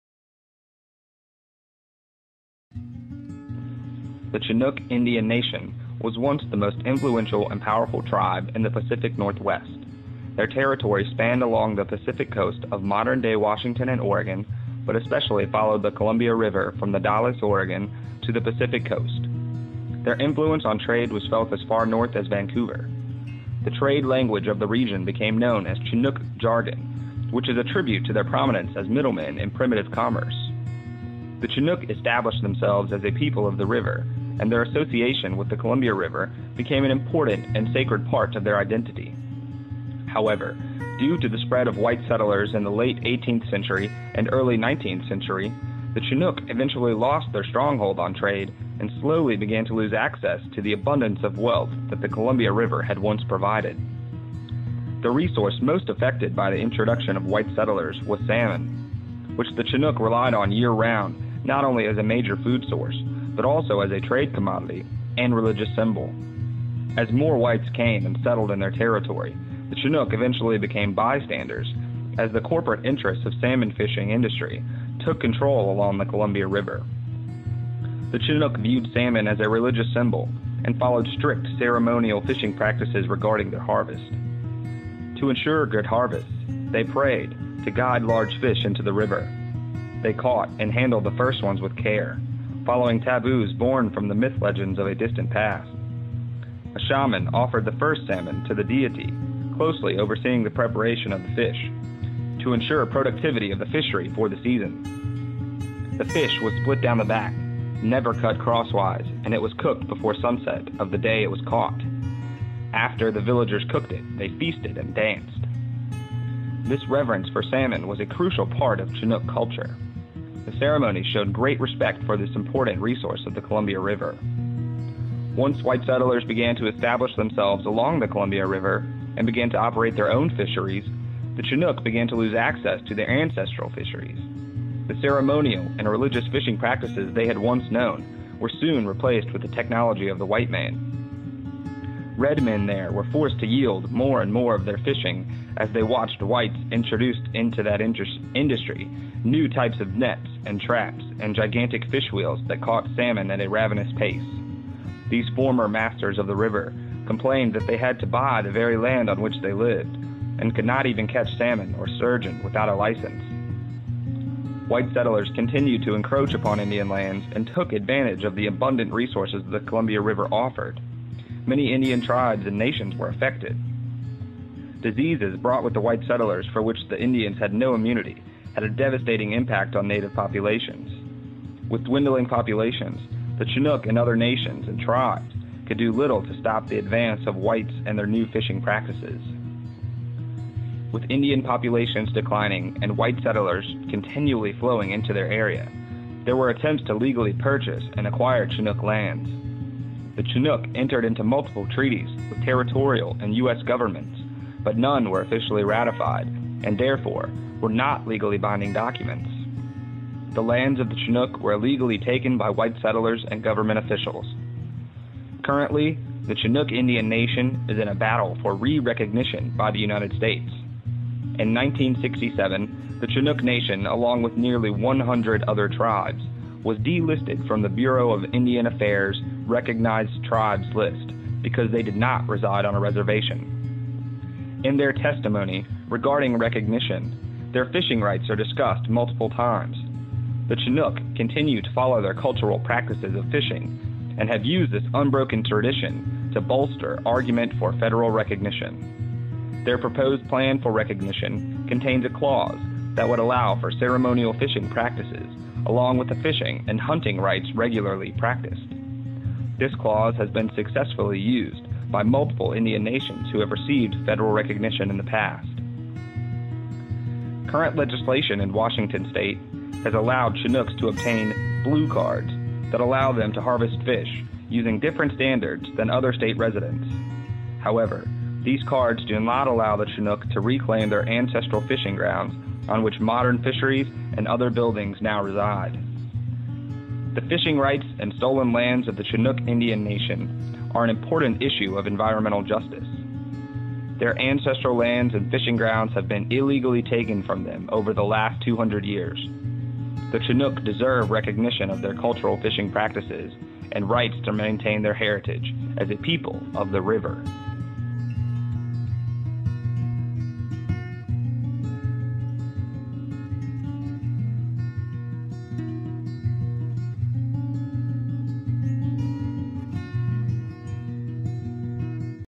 Chinook Salmon Fishing
The Chinook Indian Nation resides in Washington and Oregon. They have lost rights to ancestral lands and cultural fishing practices.

This video was created for the ANTH 317 North American Indian Cultures class during the Fall 2011 semester at the University of South Carolina.